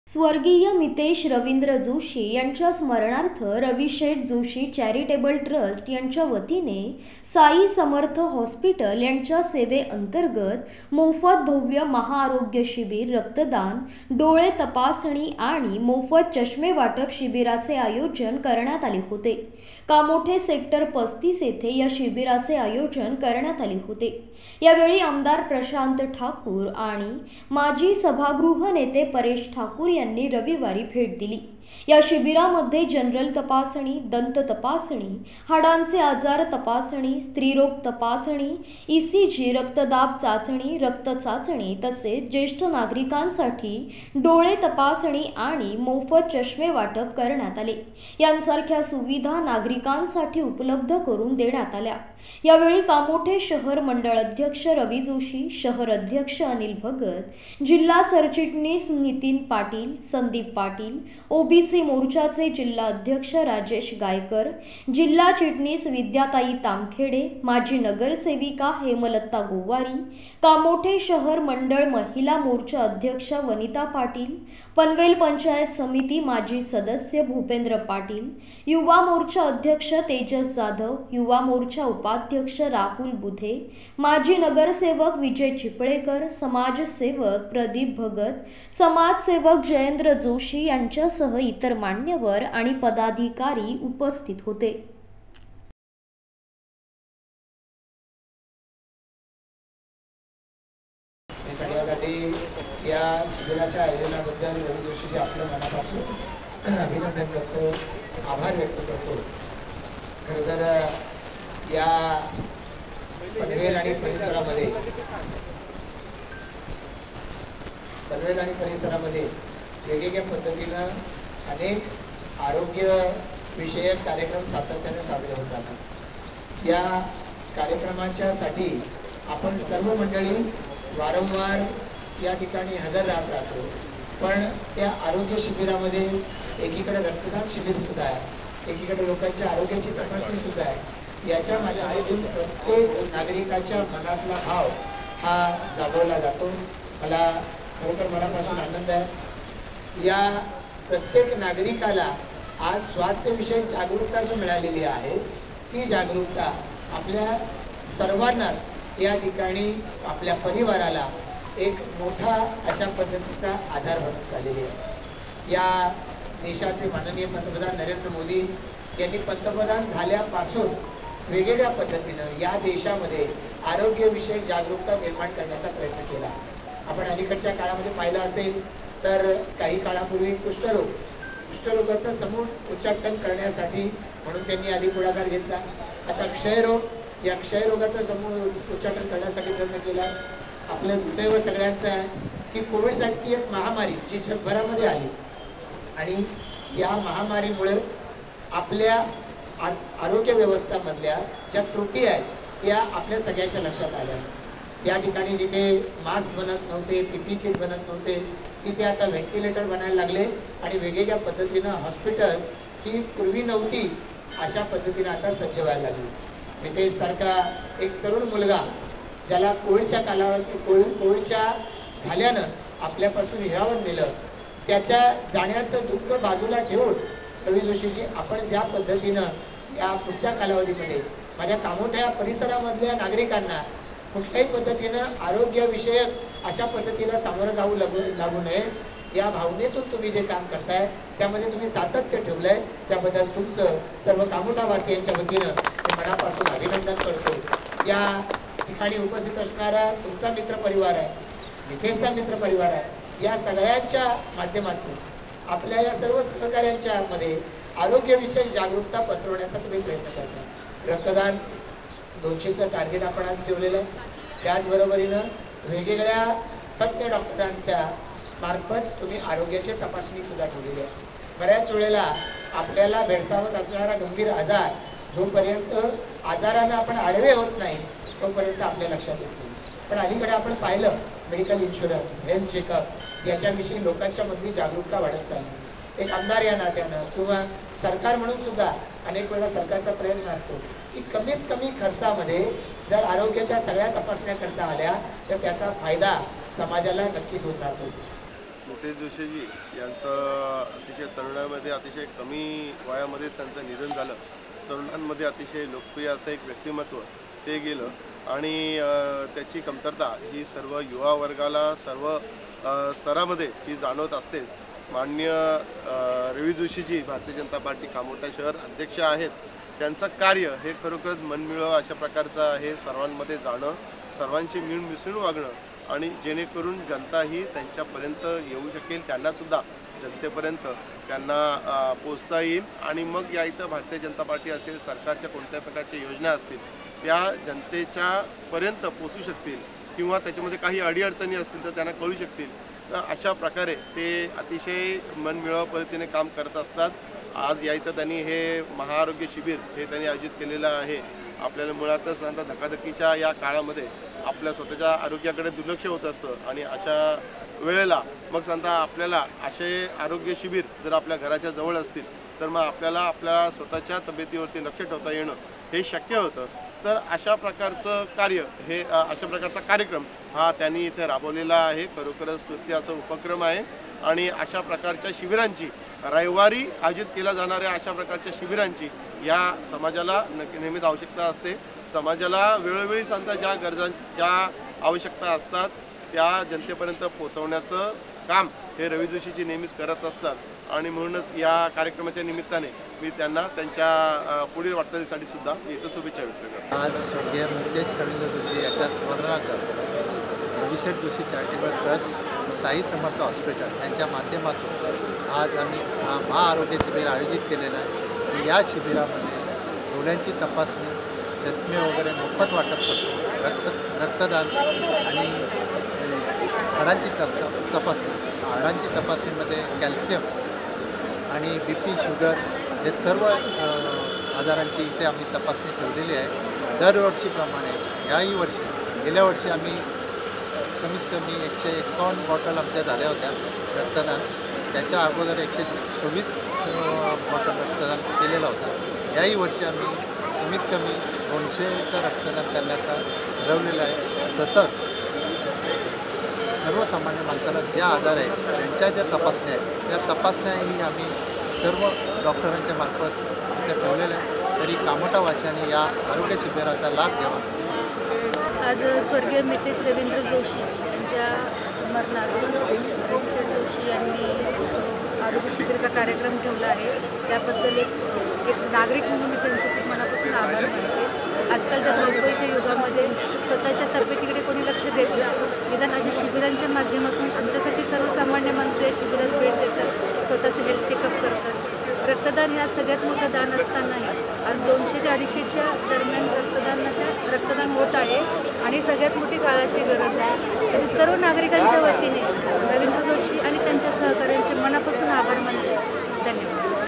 स्व. मितेश रवींद्र जोशी यांच्या स्मरणार्थ रवीशेठ जोशी चॅरिटेबल ट्रस्ट यांच्या वतीने महाआरोग्य शिबीर
स्वर्गीय मितेश रवींद्र जोशी यांच्या स्मरणार्थ रवीशेठ जोशी चॅरिटेबल ट्रस्ट यांच्या वतीने मोफत भव्य महाआरोग्य शिबीर#malharnews